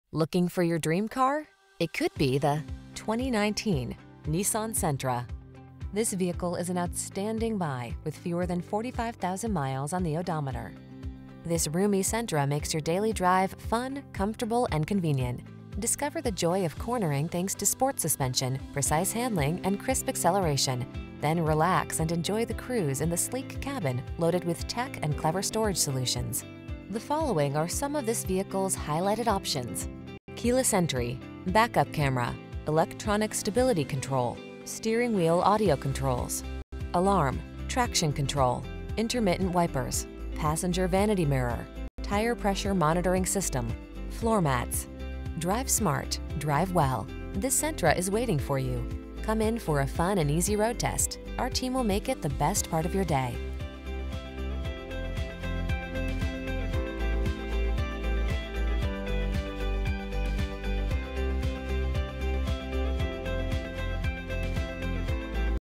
2019 Nissan Sentra S Chicago, Matteson, Oak Lawn, Orland Park, Countryside IL
Pre-Owned 2019 Nissan Sentra S available at Western Avenue Nissan located in 7410 S Western Ave, Chicago, IL, 60636.

Servicing the Chicago, Matteson, Oak Lawn, Orland Park, Countryside area.

Looking for your dream car? It could be the  2019  Nissan Sentra.  This roomy Sentra makes your daily drive fun, comfortable, and convenient. Discover the joy of cornering thanks to sport suspension, precise handling, and crisp acceleration, then relax and enjoy the cruise in the sleek cabin loaded with tech and clever storage solutions. The following are some of this vehicles highlighted options: Keyless Entry, Back-Up Camera, Electronic Stability Control, Steering Wheel Audio Controls, Alarm, Traction Control, Intermittent Wipers, Passenger Vanity Mirror, Tire Pressure Monitoring System, Floor Mats. Drive smart. Drive well. This Sentra is waiting for you. Come in for a fun and easy road test. Our team will make it the best part of your day.